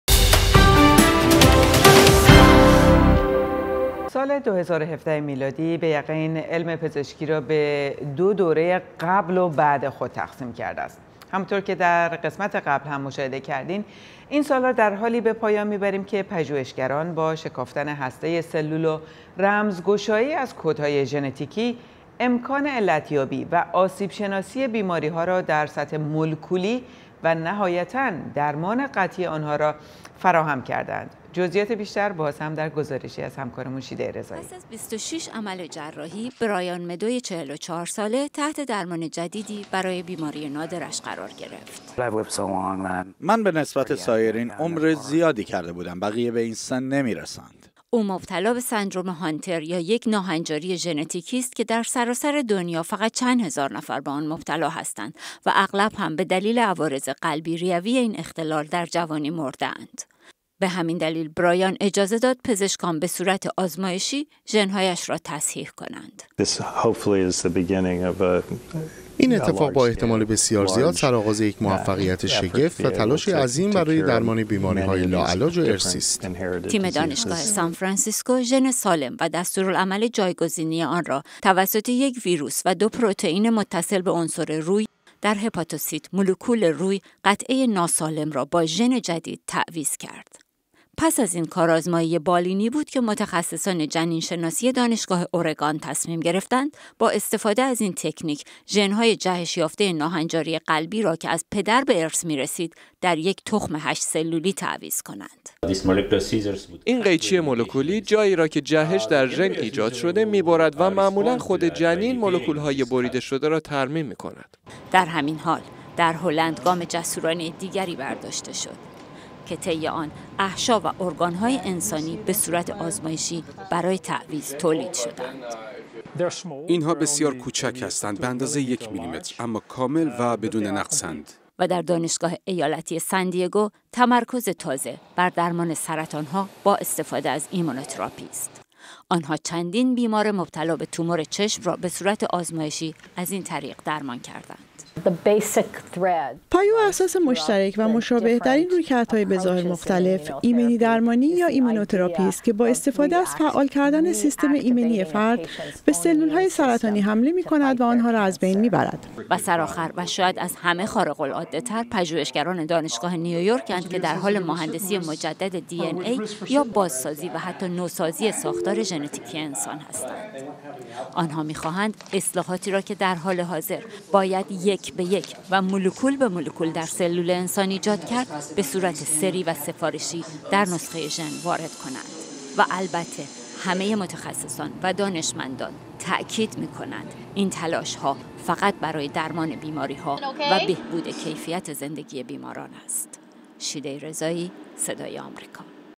یکی از مهمترین اتفاقات سال ۲۰۱۷: شکافتن هسته سلول و رمزگشایی از کدهای ژنتیکی
سال ۲۰۱۷ میلادی، به یقین، علم پزشکی را به دو دوره قبل و بعد خود تقسیم کرده است. این سال را در حالی به پایان می بریم که پژوهشگران با شکافتن هسته سلول و رمزگشایی از کدهای ژنتیکی، امکان علت یابی و آسیب شناسی بیماری ها را در سطح مولکولی، و نهایتا درمان قطعی آن ها برداشته اند.  گزارش شیده رضایی